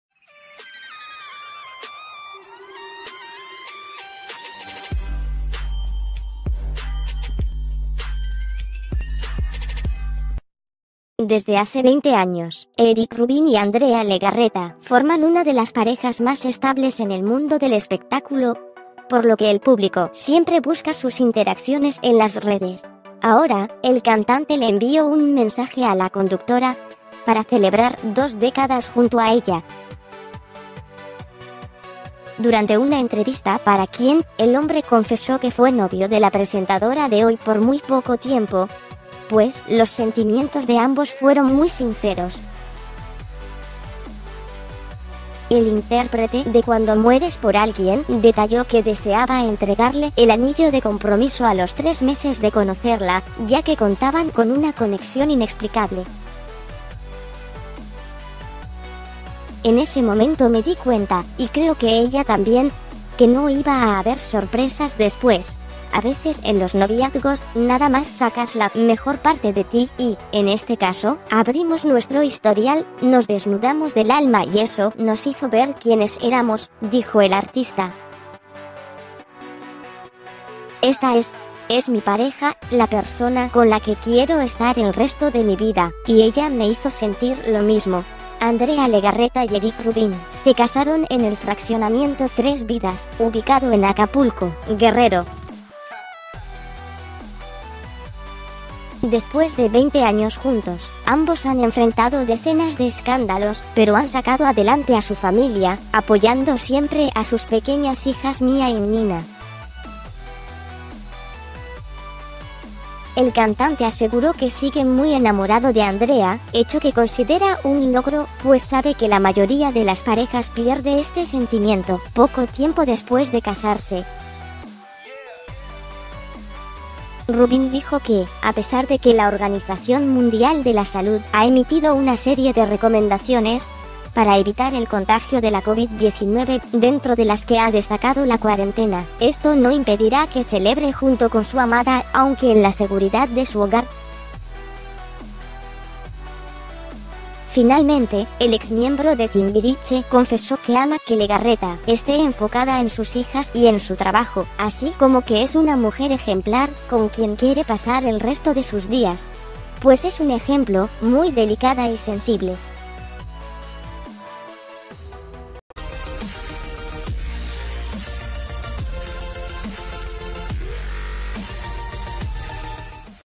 El mensaje de Erik Rubín para celebrar 20 años junto a Andrea Legarreta
Desde hace 20 años, Erik Rubín y Andrea Legarreta forman una de las parejas más estables en el mundo del espectáculo, por lo que el público siempre busca sus interacciones en las redes. Ahora, el cantante le envío un mensaje a la conductora para celebrar dos décadas junto a ella.